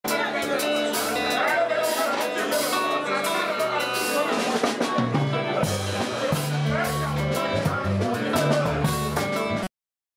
Litakwé, color bass & Pierre Didy tchakounté.MTS
Pierre Didy Tchakounté est en direct du Periph'nord à la Porte de Clignancourt avec à la percussion LITAKWE et Color Bas.